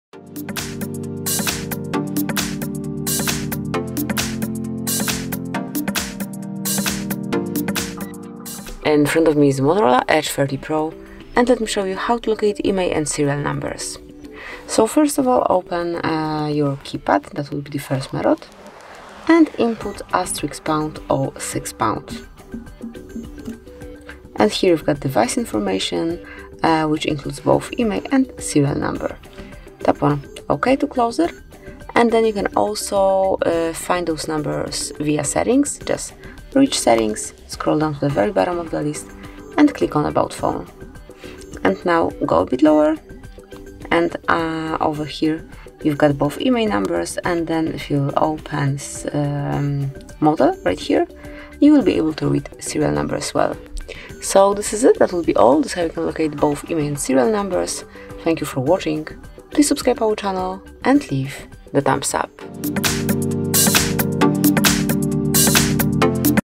How to Find IMEI and Serial Number on MOTOROLA Edge 30 Pro - Locate IMEI and Serial Number
Find out more info about MOTOROLA Edge 30 Pro:
If you want to know how to check IMEI and Serial Number on MOTOROLA Edge 30 Pro, watch this tutorial. Our specialist will show you what secret code you should enter in your MOTOROLA Edge 30 Pro so that these two unique numbers appear on your screen. We will also show you how to enter the full specifications of this device so that you can find out more about your amazing smartphone. If you have more questions about MOTOROLA Edge 30 Pro, go to our YouTube channel and watch the tutorial created by our specialists.